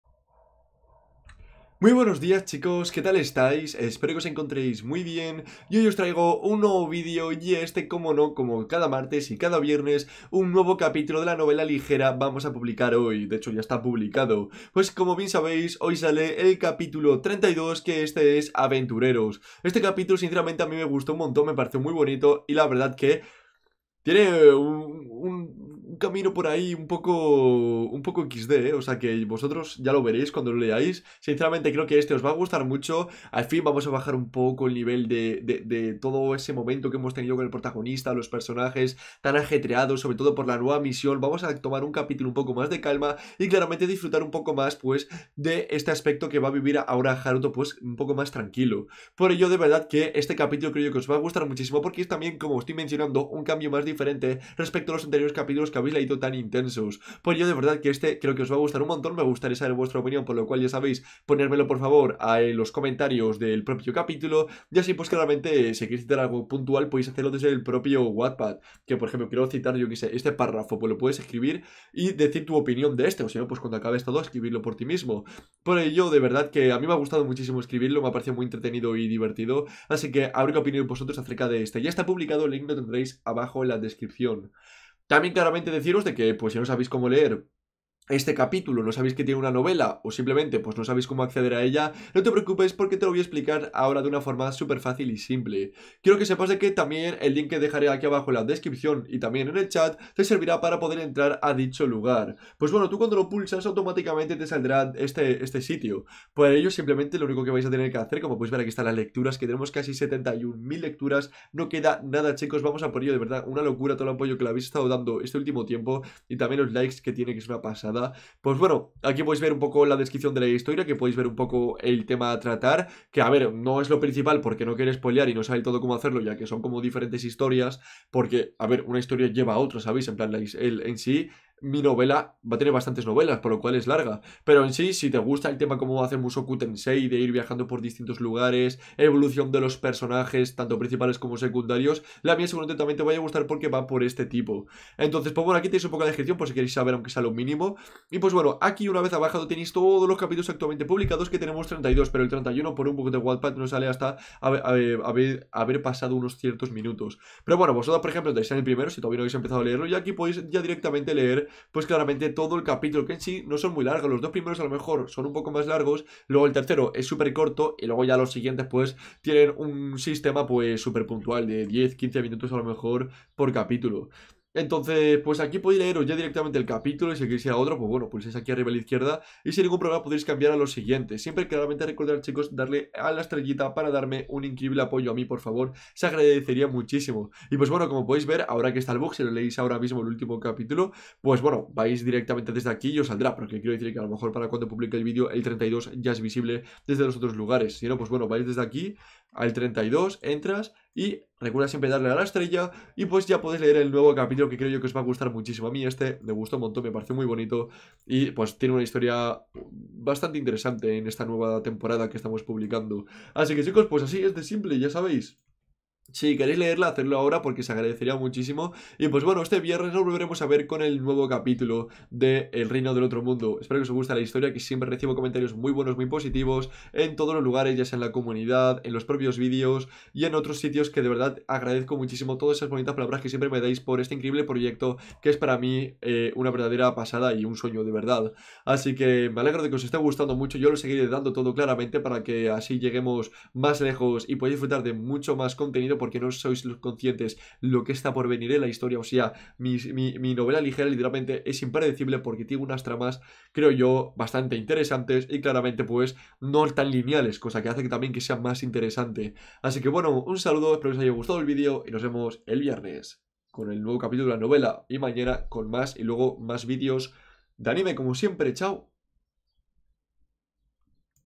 ¿ALGUIEN HA PEDIDO UN CAPÍTULO uwu? | EL REINO DEL OTRO MUNDO Cap. 32
Mi nombres es Haruto, por sucesos que me han ocurrido en el pasado no me siento muy cómodo relacionándome con otras personas, por eso prefiero evitarlas y mantener mi buena imagen personal, pero por algún motivo a una chica en concreto, todo cambia drásticamente y mis ojos solo pueden fijarse en ella...

No entiendo que ha sucedido en mi vida, esta era perfecta estando a su lado pero finalmente algo nos separó y terminé en un mundo completamente nuevo para mis ojos.

Se dice que hace más de cien años hubo una guerra tan dura que la diosa Hikari mato al Héroe y al Rey Demonio y desde entonces los dos reinos se han mantenido en silencio evitando que las personas destacaran sobre el resto hasta un cierto punto acabando con ellas para evitar sospechas y que la diosa no acabe con su reino.

Sinceramente, todo es muy raro, yo solo quiero encontrar a Elisa-san para volver a estar con ella de nuevo y ser tan feliz como lo era antes de entrar a este nuevo mundo...

Una historia original de victoruch.